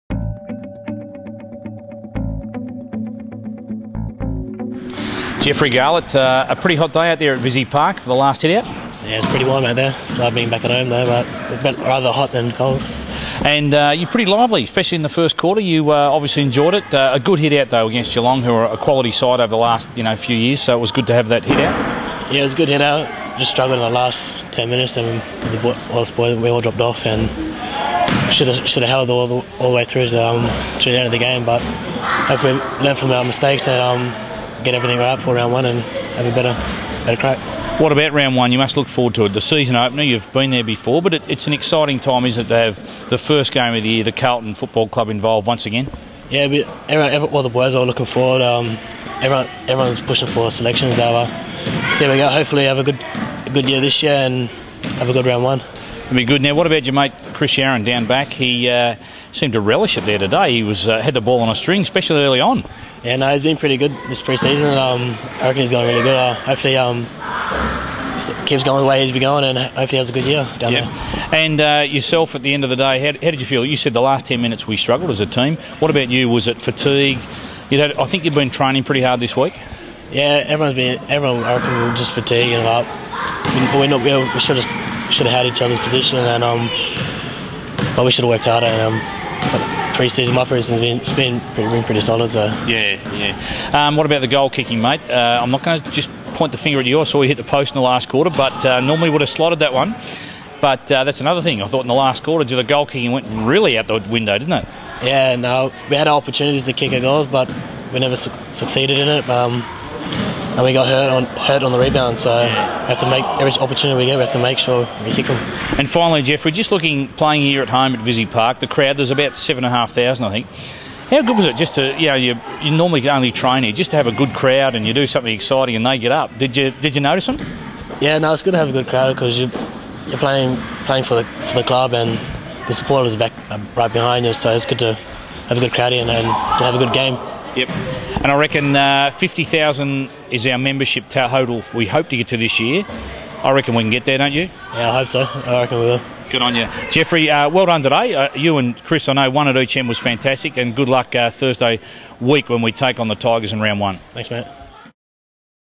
Jeff Garlett Post Match
Jeff Garlett spoke to CFC TV following the NAB Challenge match against Geelong.